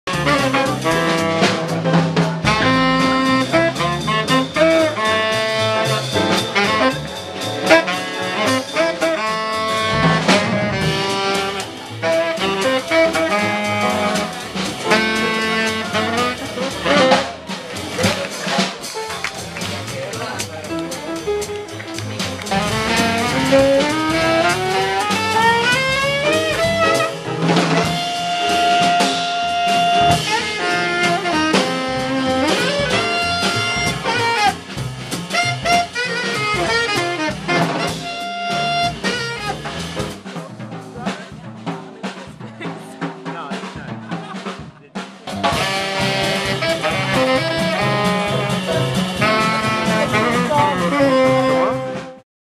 HOT CLUB
Vídeo realizado por Helena Díaz-Palacios Sevilla.

HOT CLUB DE GAND es un bar --taberna belga-- en el que cada noche de domingo organizan conciertos de Jazz (¡Gratis!). Para llegar a él, hay que atravesar un estrecho y pintoresco callejón --de noche impregnado con tintes amarillos por la luz de las farolas--. Allí van belgas y gran cantidad de erasmus, ya que cualquier extranjero que se precie no puede hacer otra cosa más que enamorarse del lugar, y de sus músicos.